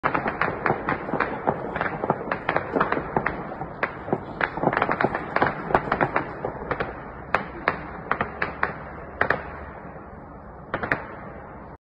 КИЕВ
СНОВА ВОЗДУШНЫЙ БОЙ С ДРОНОМ.